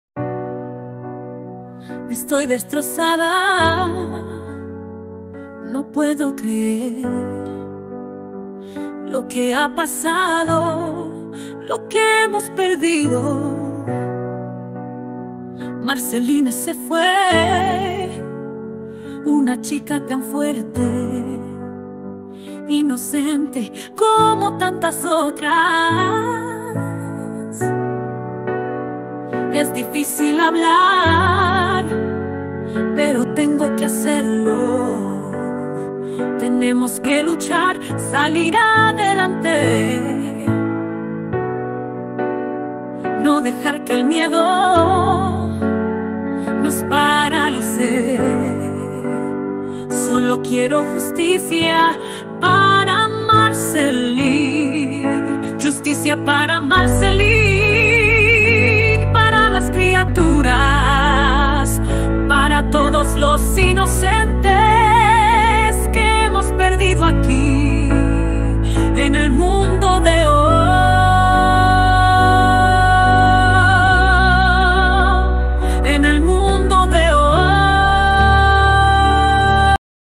(Verse 1) (6)
power pop folk Female Voice

V2
17 de marzo de 2024

(Verse 1)
Estoy destrozada, no puedo creer
Lo que ha pasado, lo que hemos perdido
Marceline se fue, una chica tan fuerte
Inocente, como tantas otras

Es difícil hablar, pero tengo que hacerlo
Tenemos que luchar, salir adelante
No dejar que el miedo nos paralice
Solo quiero justicia, para Marceline

(Chorus)
Justicia para Marceline, para las criaturas
Para todos los inocentes que hemos perdido aquí
En el Mundo de Ooo, en el Mundo de Ooo

(Verse 2)
Yo la ayudé, con su currículum
Quería un trabajo, salir adelante
Marceline luchaba, como músico independiente
Pero apenas le alcanzaba, y ahora esto

Cruel e injusto, su destino sellado
Justo cuando empezaba, un nuevo camino
En el escenario, brillando su luz
Ahora nos quedamos, en la oscuridad

(Bridge)
Vivir con miedo, en el Mundo de Ooo
Atados de manos, pensando dos veces
Una realidad normalizada, pero